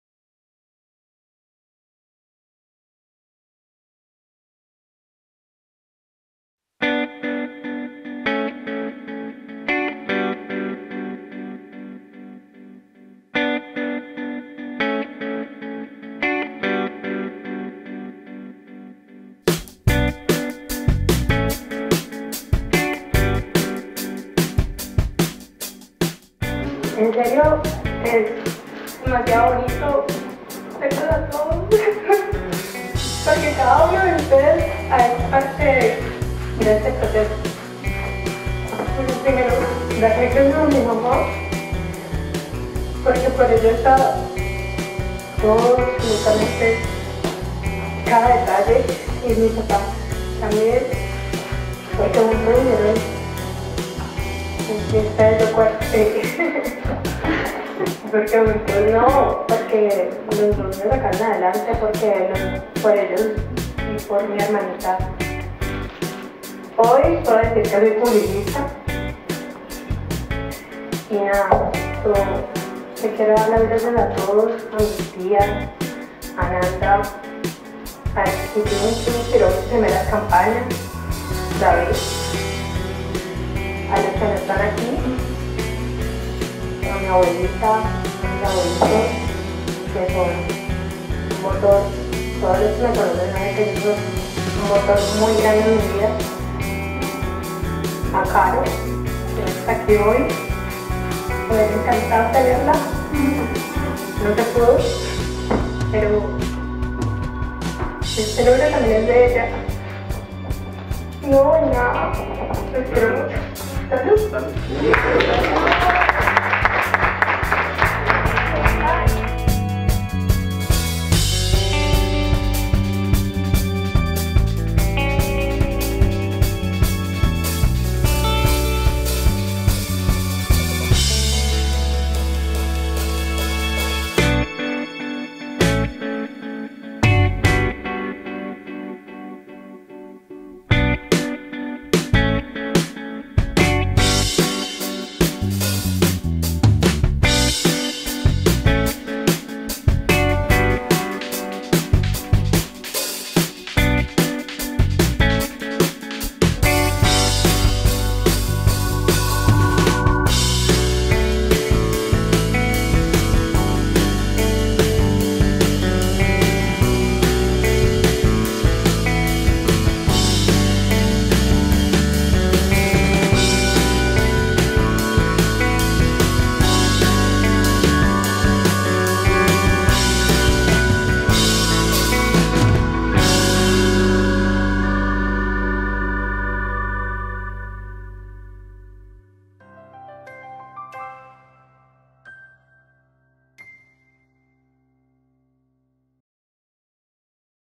CLAUDIA ALVAREZ SALON PIEDRA GRANDE (TRAILER)
Iniciando el mes de Octubre con sorpresas y una de ellas es este gran vídeo con el apoyo de la CASA DE EVENTOS PIEDRA GRANDE Y LA PELUQUERÍA FERNANDO CASTILLO

FELIZ GRADO : CLAUDIA
AHORA ERES UNA GRAN PUBLICISTA

FRANKLIN FOTOGRAFÍA: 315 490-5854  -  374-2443 (CALI)